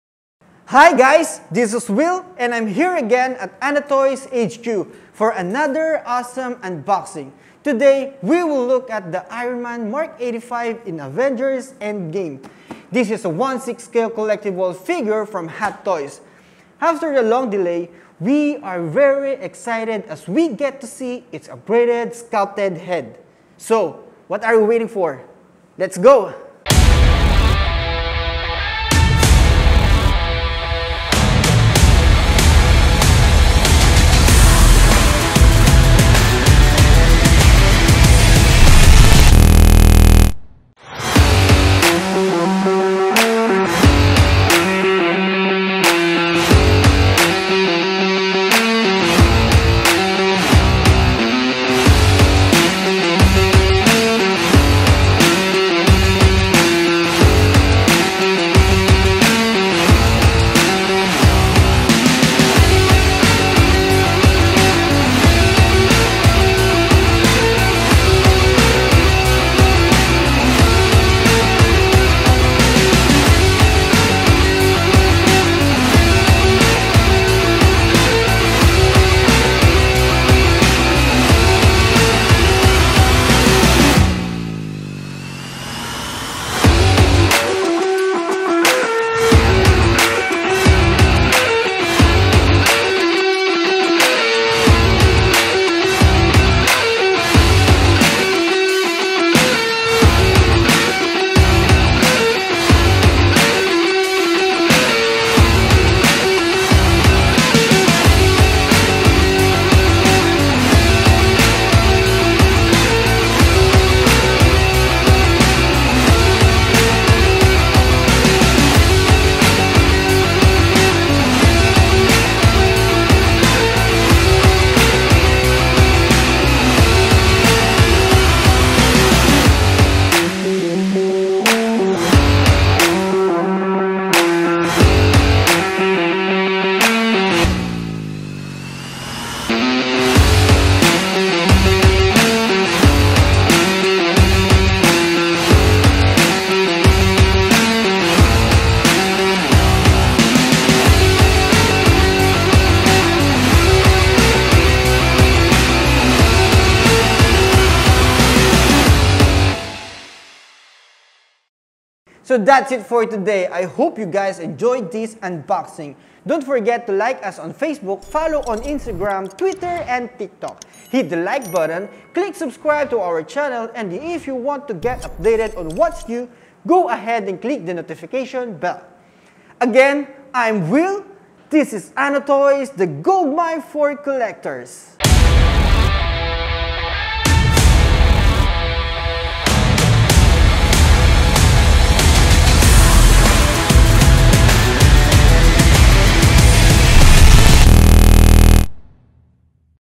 Quick Unboxing: Hot Toys Sixth Scale Avengers Endgame Iron Man Mark 85 | MMS528D30
Exquisitely crafted based on Tony Stark/Iron Man in the Avengers: Endgame, the true-to-movie figure features two interchangeable head sculpts including a newly developed head sculpt with remarkable likeness and an interchangeable helmeted head with LED-light up function, intricate design of the Iron Man armor has been faithfully reproduced with red, gold and charcoal grey coloring painted carefully with a metallic appearance, LED light-up functions scattered throughout the upper body, back and Arc Reactor on chest, Nano Lightning Refocuser attachable to the back, multiple pairs of interchangeable hands for iconic gestures, character themed figure stand with movie logo.

Today let’s unveil this incredible Iron Man Mark 85 by Hot Toys only here at Anotoys Collectibles, the goldmine for collectors.